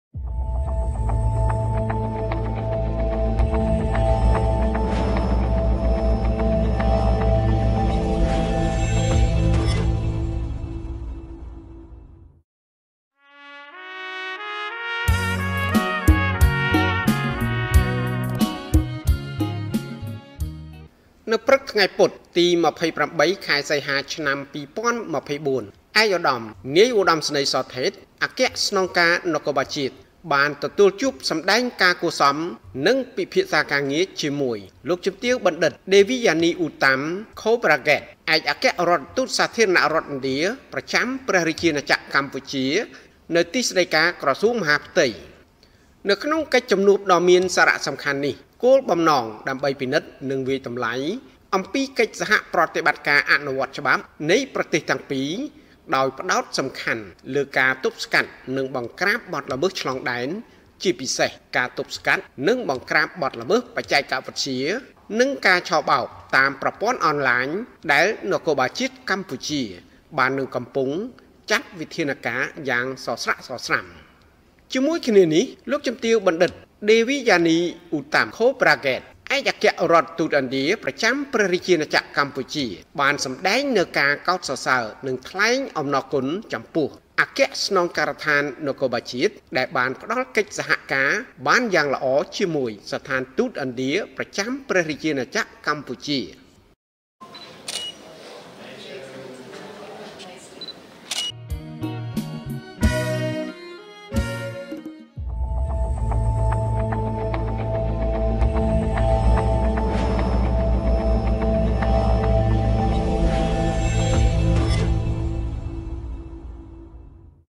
ឯ.ឧ ស ថេត ជួបសម្តែងការគួរសម និងពិភាក្សាការងារជាមួយឯកអគ្គរដ្ឋទូតឥណ្ឌា
រាជធានីភ្នំពេញ ៖ ព្រឹកថ្ងៃពុធ ទី២៨ ខែសីហា ឆ្នាំ២០២៤នេះ ឯកឧត្តម នាយឧត្តមសេនីយ៍ ស ថេត អគ្គស្នងការនគរបាលជាតិ បានទទួលជួបសម្តែងការគួរសម និងពិភាក្សាការងារជាមួយលោកជំទាវបណ្ឌិត ដេវីយ៉ានី អ៊ុតាំ ខូប្រាហ្គេត(Devyani Uttam Khobragade) ឯកអគ្គរដ្ឋទូតសាធារណៈរដ្ឋឥណ្ឌា ប្រចាំព្រះរាជាណាចក្រកម្ពុជា នៅទីស្តីការក្រសួងមហាផ្ទៃ។
នៅក្នុងកិច្ចជំនួបដ៏មានសារៈសំខាន់នេះ គោលបំណងដើម្បីពិនិត្យ និងវាយតម្លៃអំពីកិច្ចសហប្រតិបត្តិការអនុវត្តច្បាប់ នៃប្រទេសទាំងពីរ ដោយផ្តោតសំខាន់លើការទប់ស្កាត់ និងបង្រ្កាបបទល្មើសឆ្លងដែន ជាពិសេសការទប់ស្កាត់ និងបង្រ្កាបបទល្មើសបច្ចេកវិទ្យា និងការឆបោកតាមប្រព័ន្ធ Online ដែលនគរបាលជាតិកម្ពុជា បាននឹងកំពុងចាត់វិធានការយ៉ាងសស្រាក់សស្រាំ។
ជាមួយគ្នានេះ លោកជំទាវបណ្ឌិត ដេវីយ៉ានី អ៊ុតាំ ខូប្រាហ្គេត (Devyani Uttam Khobragade) ឯកអគ្គរដ្ឋទូតឥណ្ឌា ប្រចាំព្រះរាជាណាចក្រកម្ពុជា បានសម្តែងនូវការកោតសរសើរ និងថ្លែងអំណរគុណចំពោះអគ្គស្នងការដ្ឋាននគរបាលជាតិ ដែលបានផ្តល់កិច្ចសហការបានយ៉ាងល្អជាមួយស្ថានទូតឥណ្ឌាប្រចាំព្រះរាជាណាចក្រកម្ពុជា៕